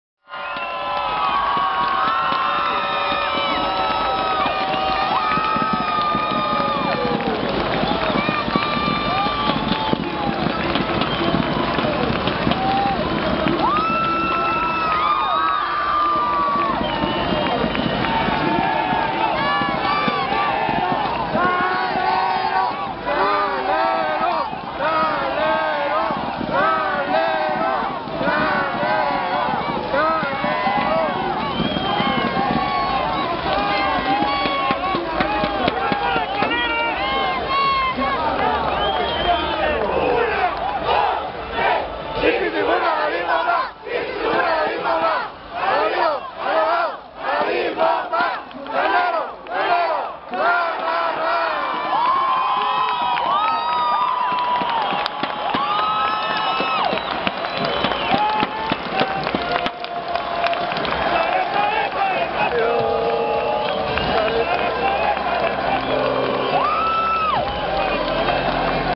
Despedida de Miguel Calero
salida de Miguel Clero en su partido de despedida, Jornada 14 del Torneo Apertura 2011 Estadio Hidalgo. Pachuca 0-Pumas UNAM Jornada 14

Créditos:Aficionado
Ituzos.com Sitio Oficial de la Afición Tuza